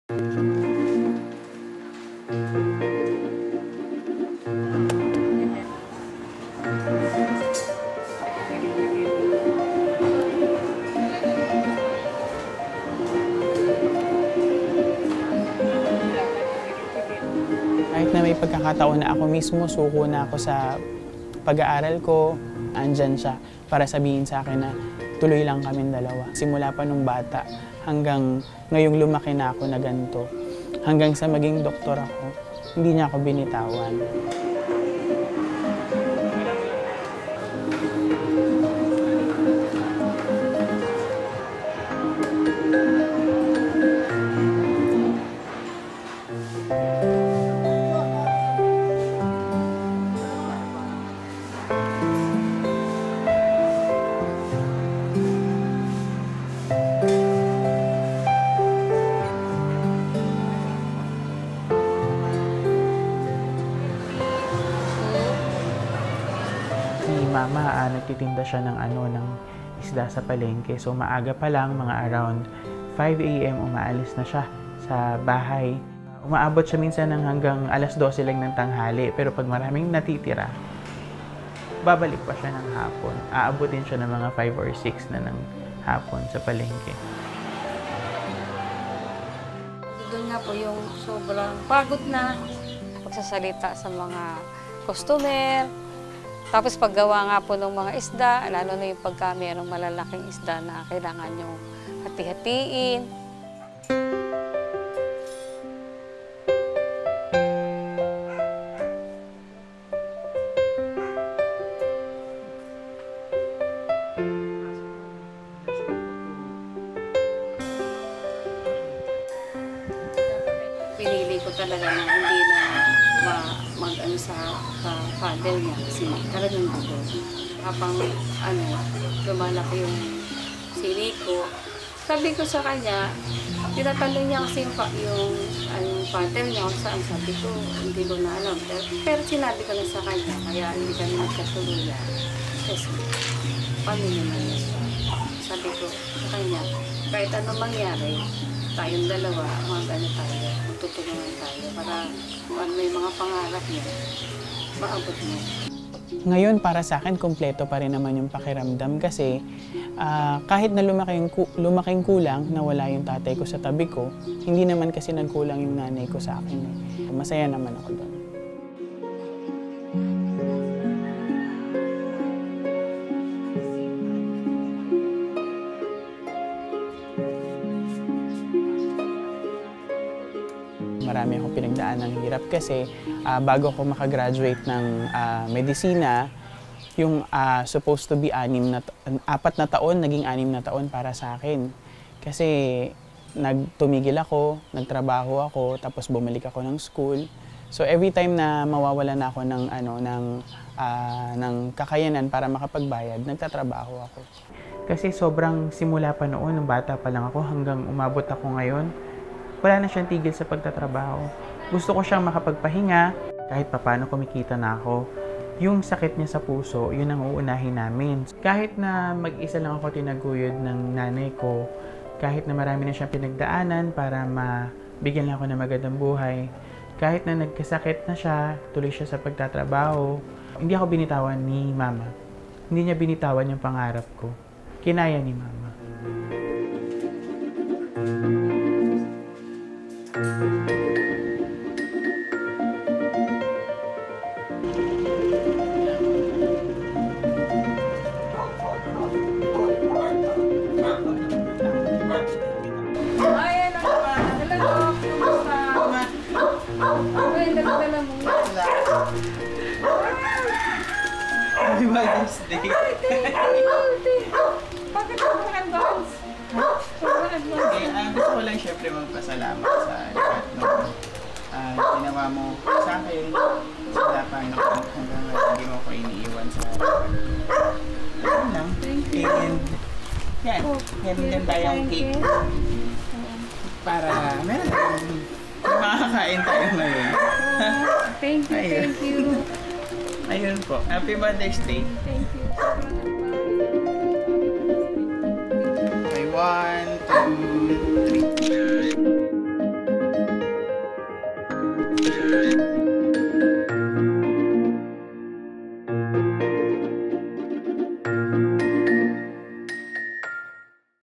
KinayaNiMama: Tindera ng isda, nakapagpatapos ng doktor!
Sa kabila ng sakit niya sa puso, araw-araw pa ring nagtitinda ng isda ang single mother na si Nanay Nanita para lang mapagtapos sa medisina ang kanyang anak. Ma-inspire at ibahagi ang kanilang kuwento!